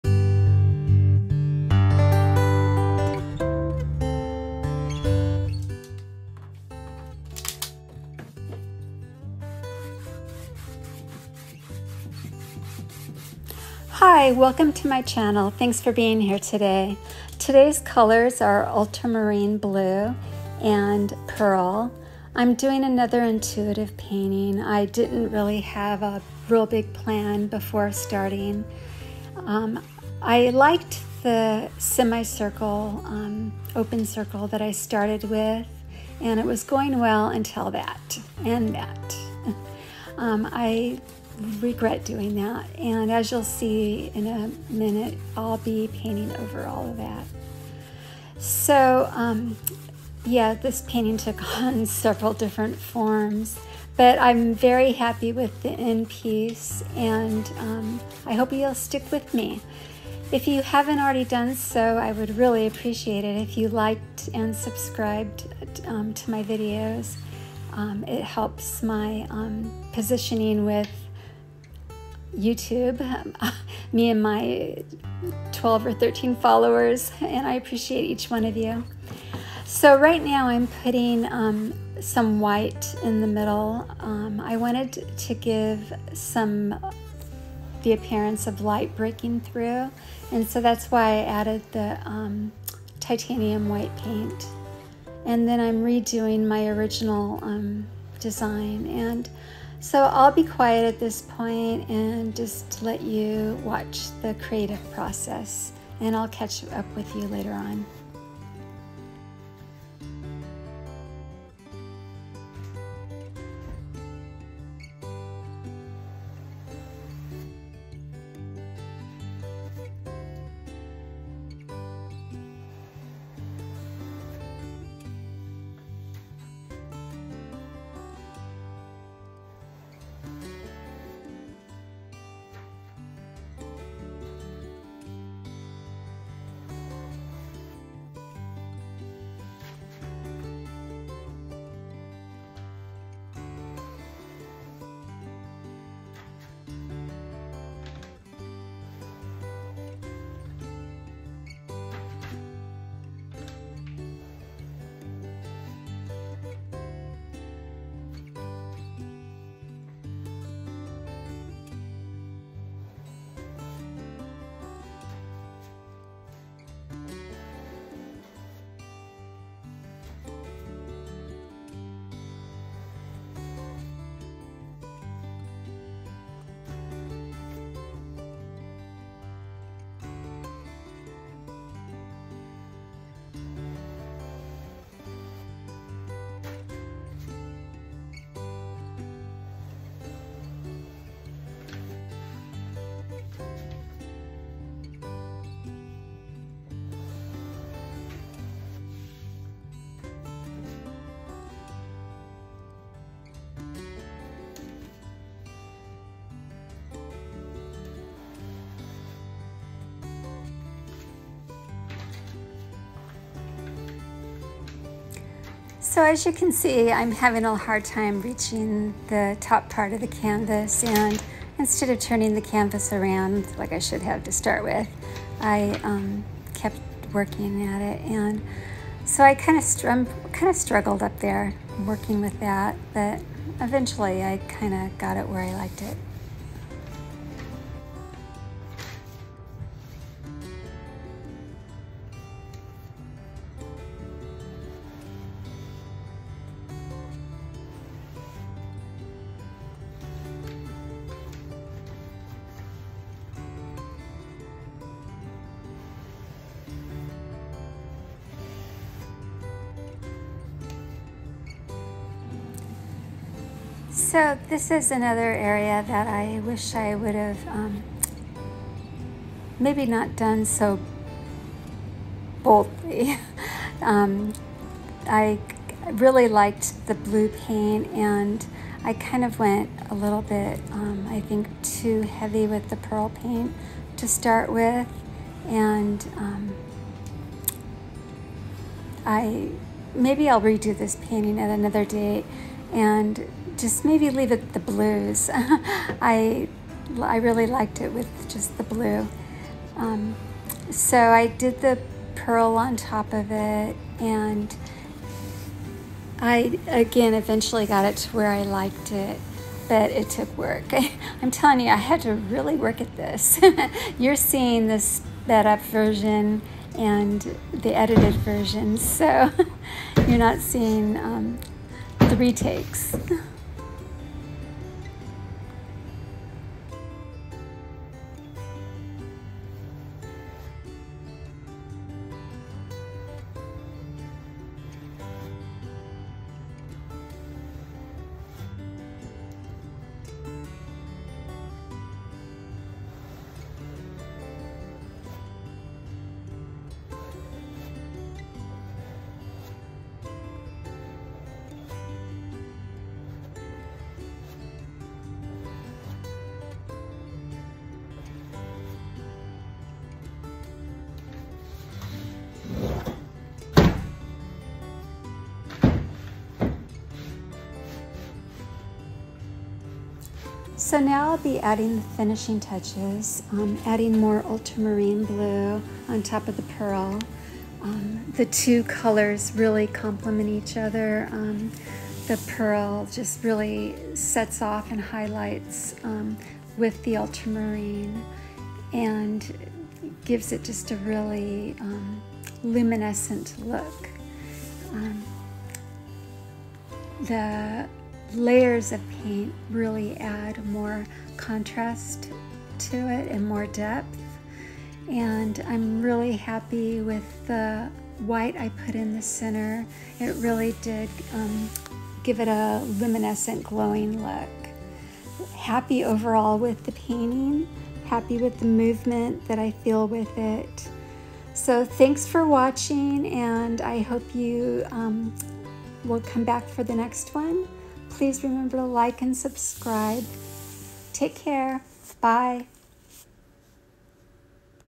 WOW…Mood painting Changes with the light/ Fluid Art with One Brush
Botanical painting using Ultramarine blue and pearl colored acrylic paints. This painting changes its mood with the light. In dimmer light it looks sultry with a romantic feel and in bright light it shines like snow and ice on a bright winters day.  The edges are painted blue black so no frame is needed.

Dimensions 20” x 24” x 2” gallery wrapped canvas

Brush used was 4” freestyle by liqutex